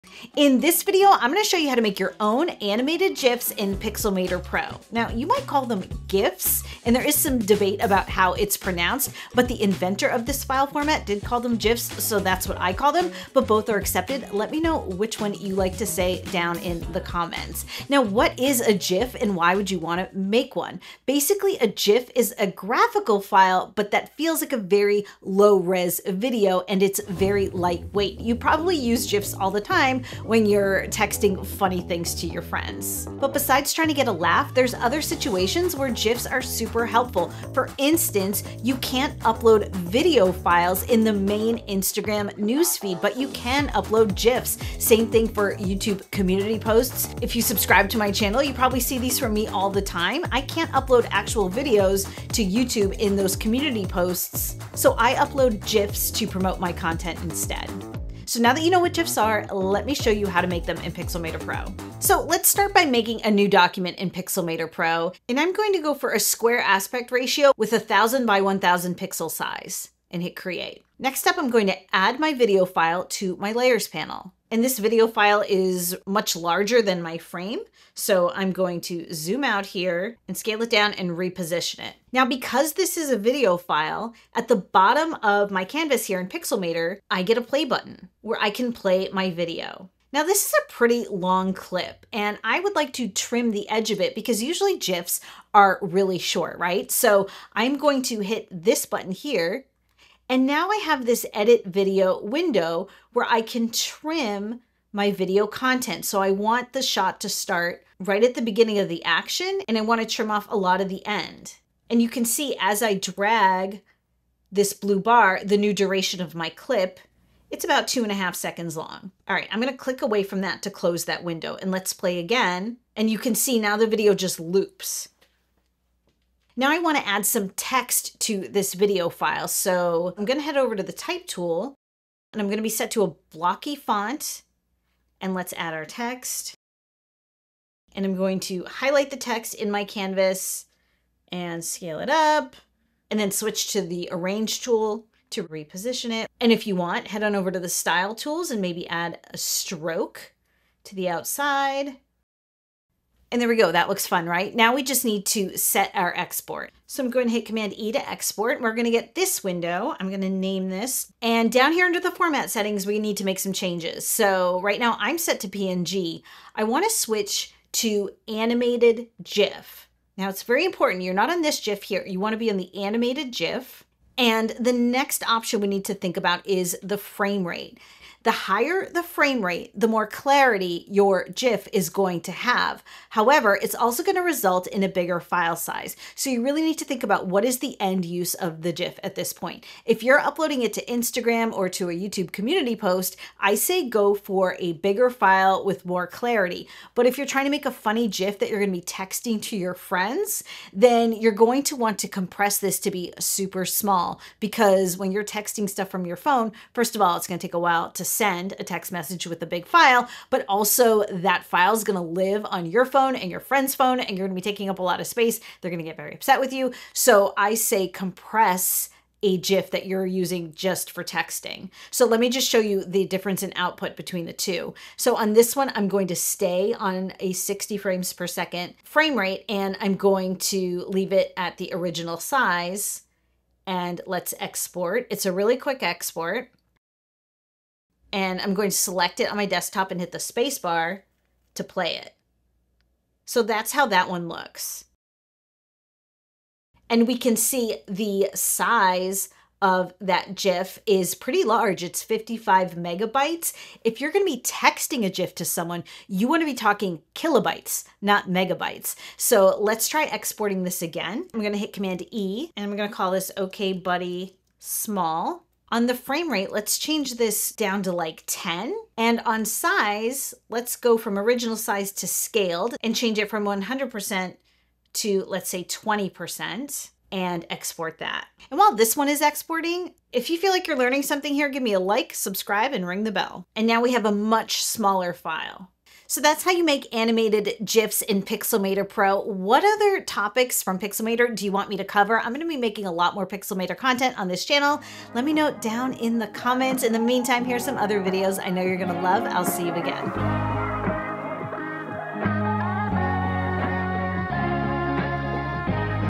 Make Your Own GIFs in Pixelmator Pro
In this Pixelmator Pro tutorial, let me show you how to create your own animated GIFs that you can text to your friends as animated memes or upload to the Instagram feed or use as Community Posts on YouTube!

00:00 Make your own GIFs in Pixelmator Pro
00:16 What is a GIF?
00:28 What are GIF files used for?
01:21 Trimming Videos in Pixelmator Pro
02:07 Adding Text over Video In Pixelmator Pro
02:36 Export GIFs in Pixelmator Pro TutorialVisit to learn more.

If you like the music and graphics from my video, check out Motion Array